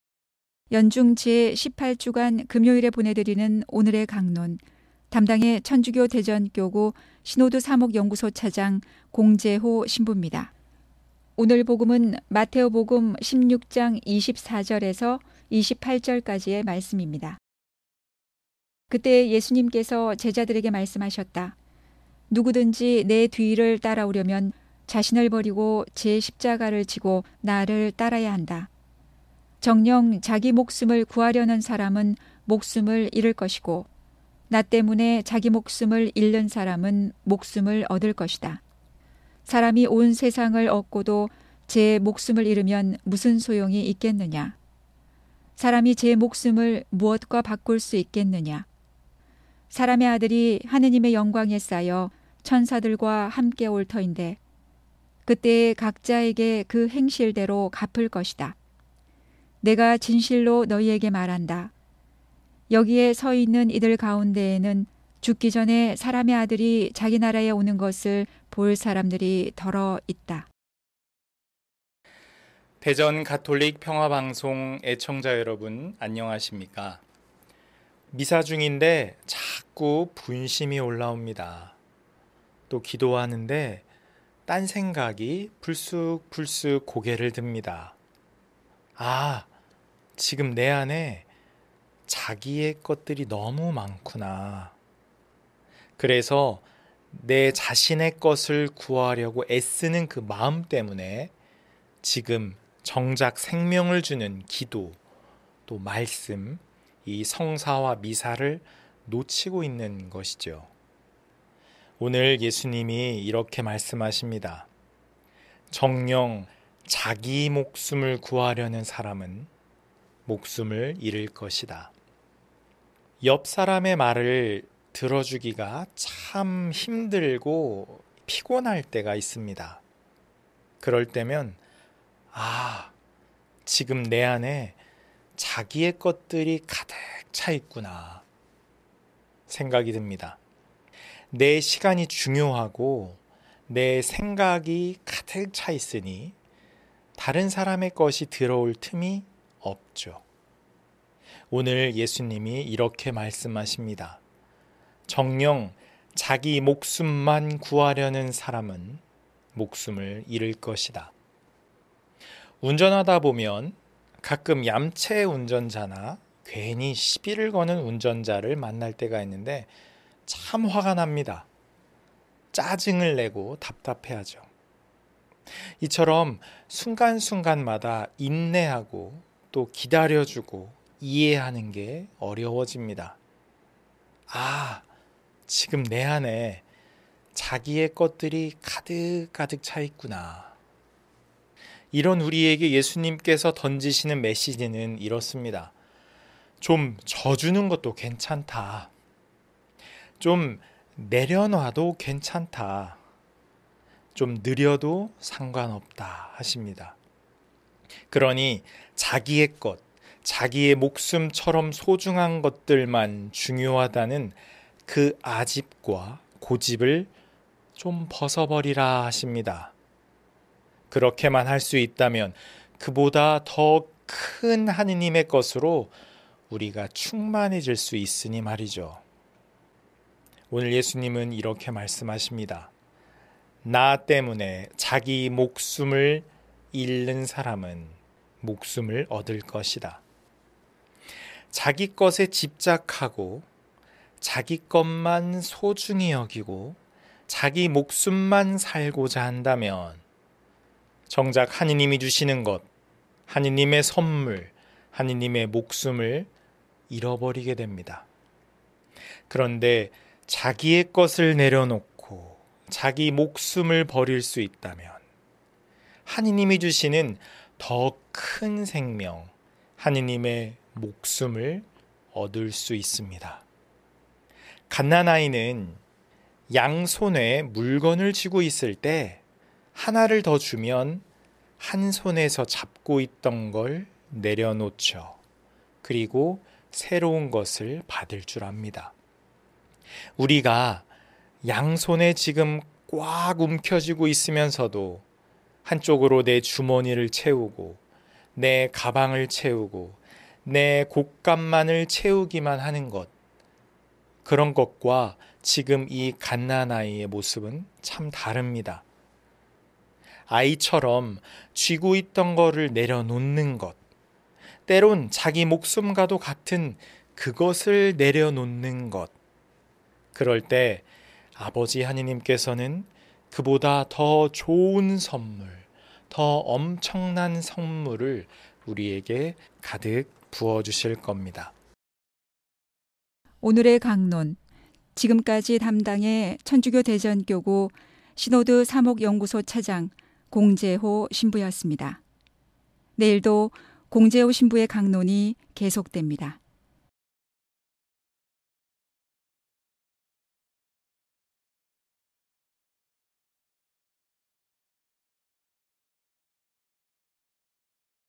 2022.08.05 오늘의 강론 공재호 신부 (금)
오늘의 강론 2022년 08월 01일(월)부터 2022년 08월 06일(토)까지 연중 제 18주간
천주교 대전교구 시노드 사목연구소 차장 공재호 신부의 강론이 방송됩니다.
많은 청취 바랍니다